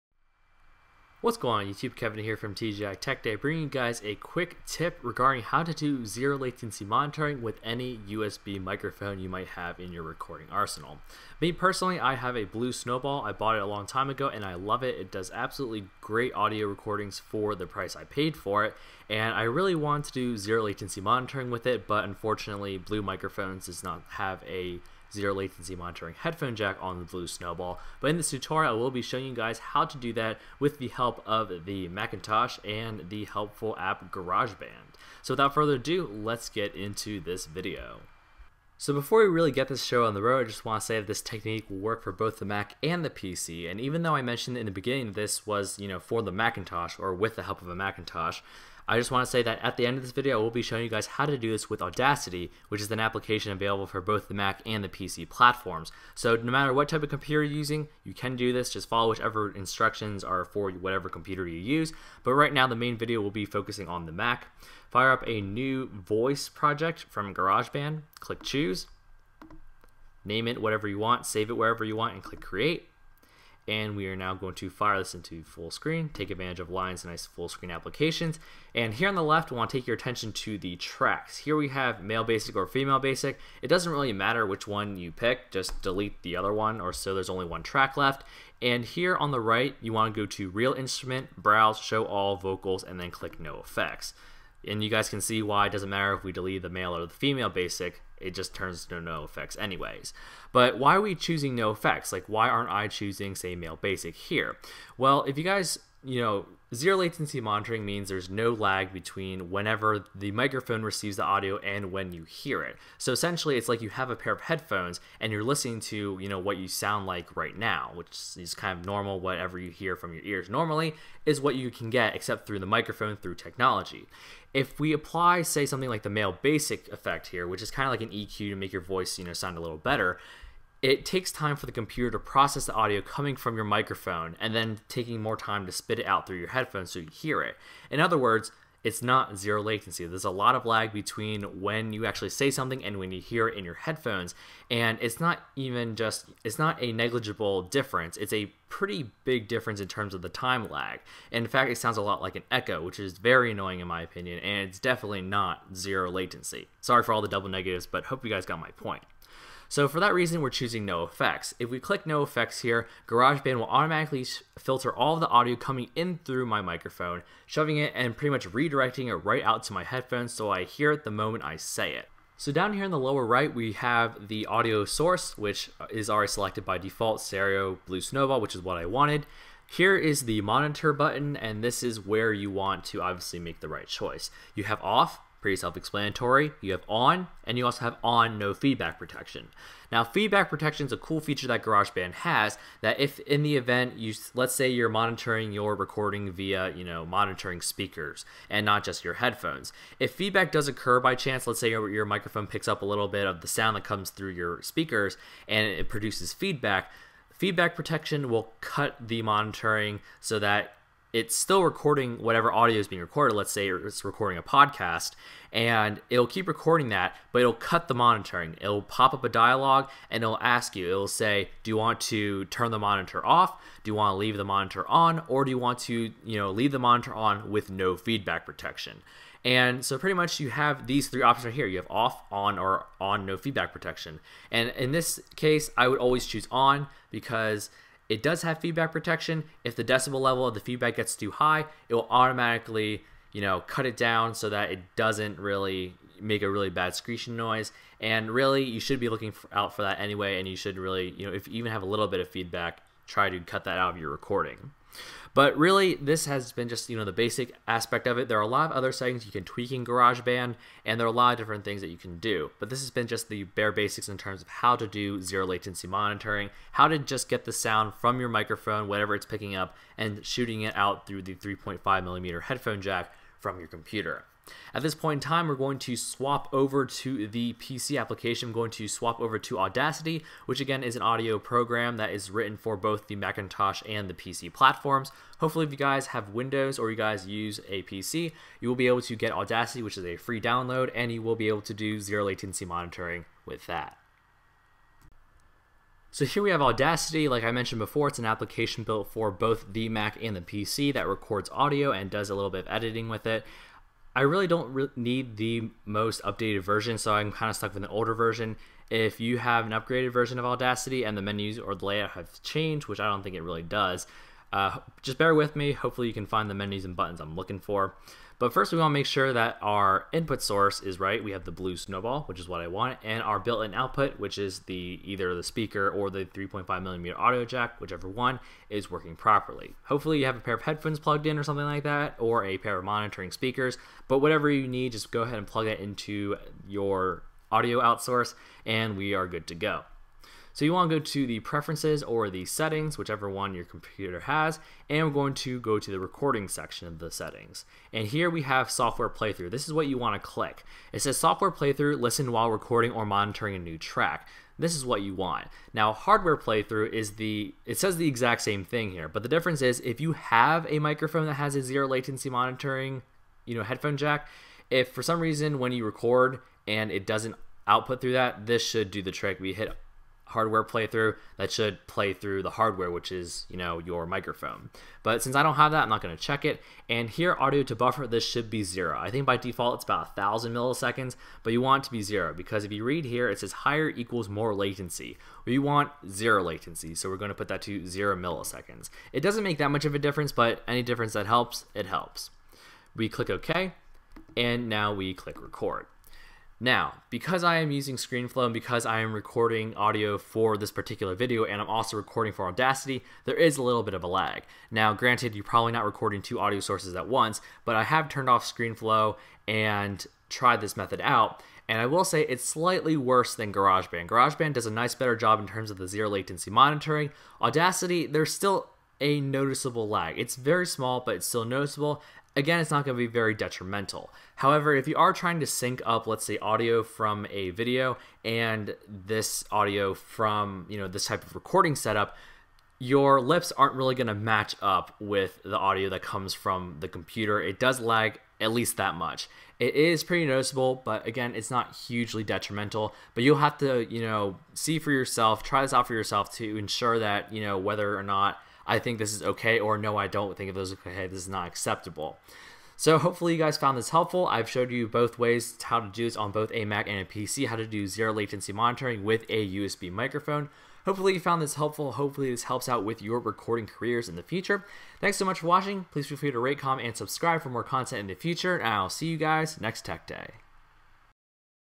Zero Latency Monitoring with the Blue Snowball (or any USB Microphone, Mac & PC)
Do you own a USB microphone that doesn't have a zero latency monitoring system? Do you want one? In this video I show a simple way to listen to your recordings with both Mac and PC-compatible applications.